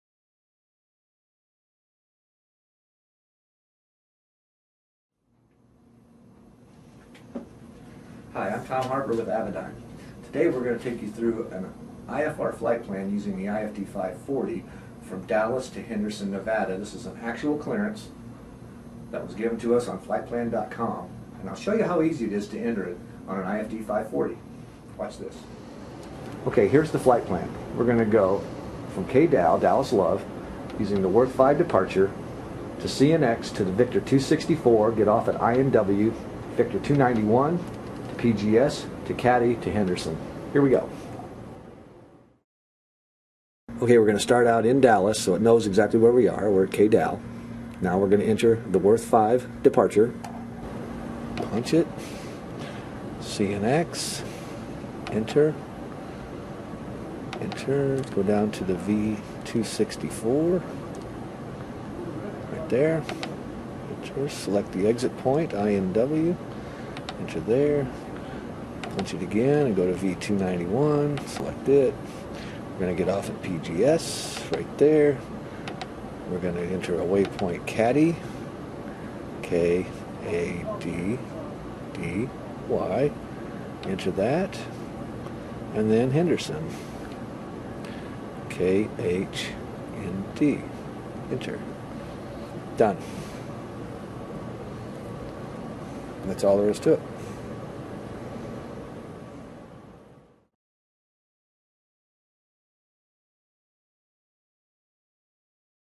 Avidyne IFD540 Easy Flight Plan Entry
Avidyne's Tom Harper takes you through the simple process of entering a flight plan on the new IFD540 FMS/GPS/NAV/COM